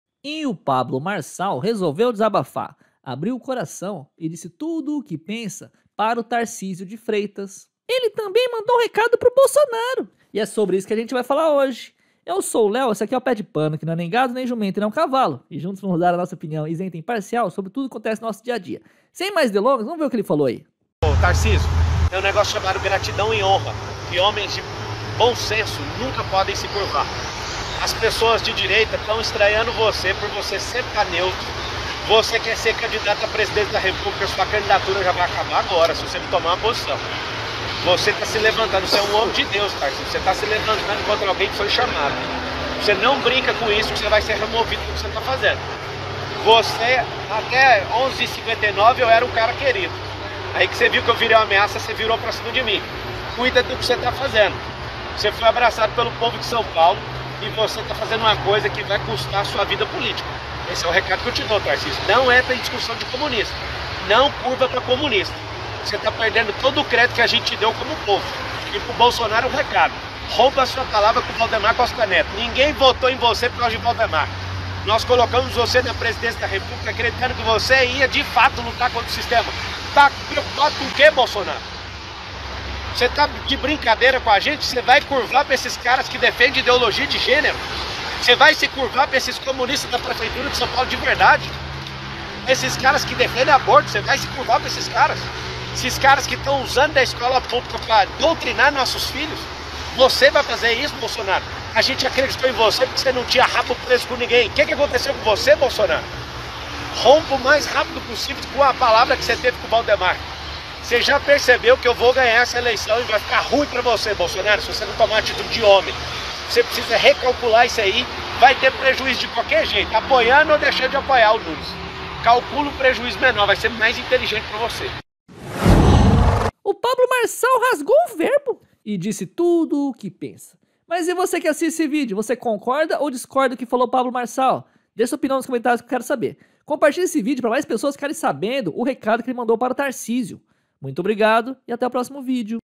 PABLO MARÇAL RESPONDE TARCISIO DE FREITAS !
Tarcisio de freitas recebe resposta de Pablo Marçal